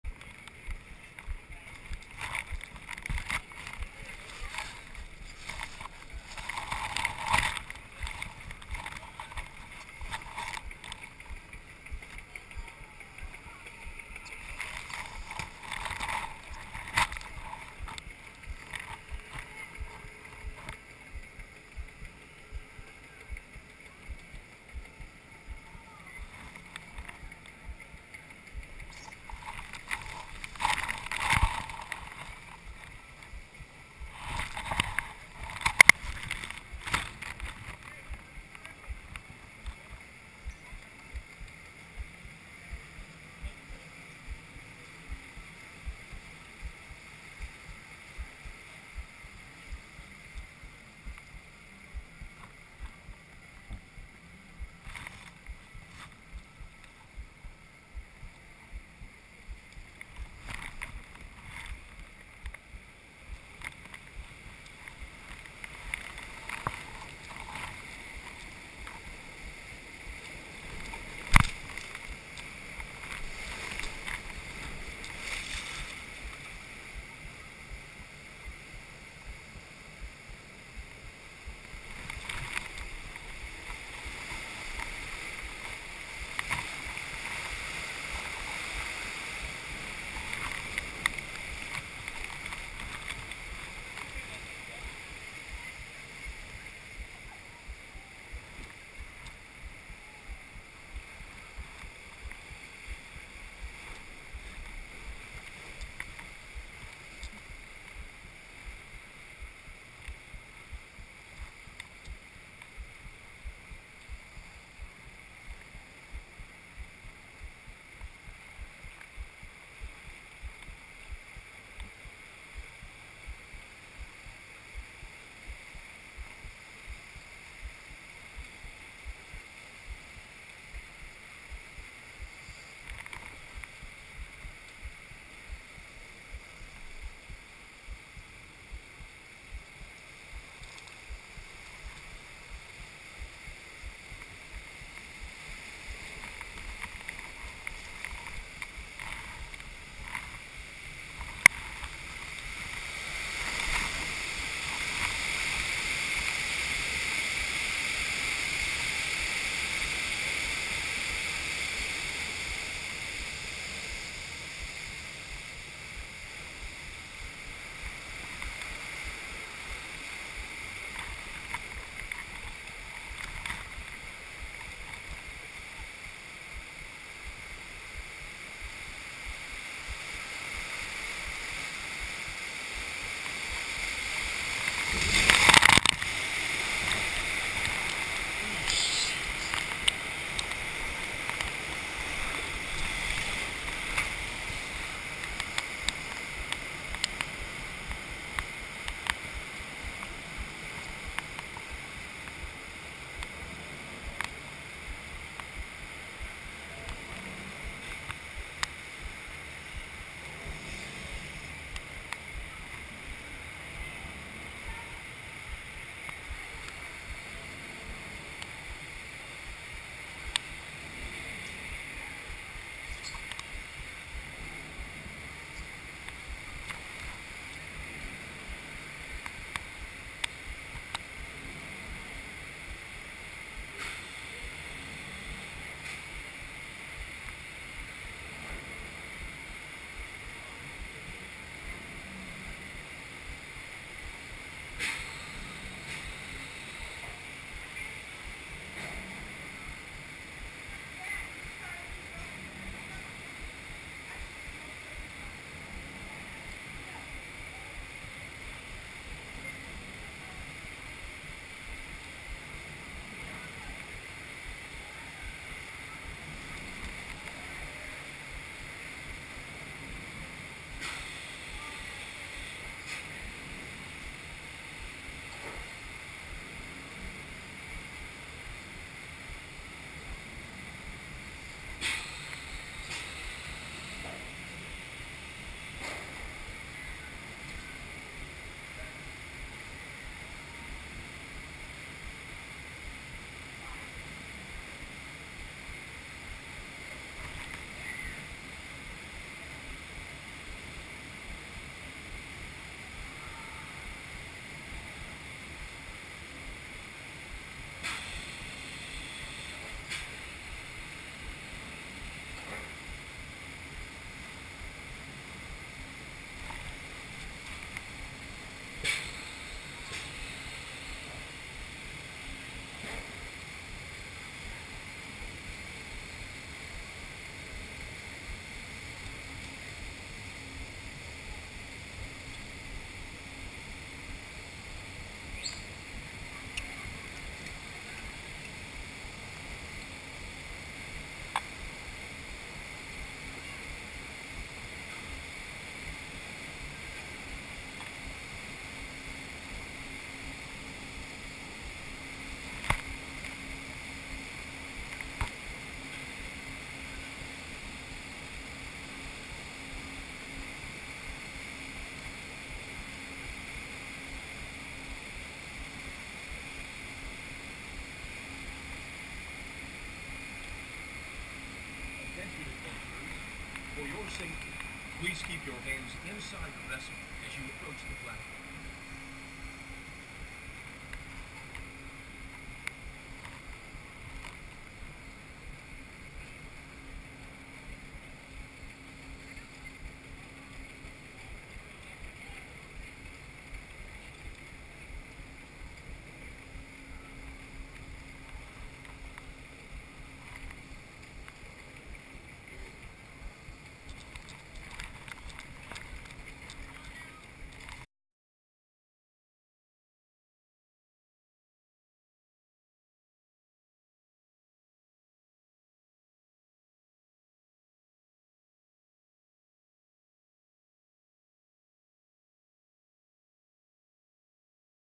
Shipwreck Rapids (HD POV) SeaWorld San Diego
This is a (HD POV) of Shipwreck Rapids at SeaWorld In San Diego, California.

Send me stuff to: 3750 Midland Drive # 149 Roy Utah, 84067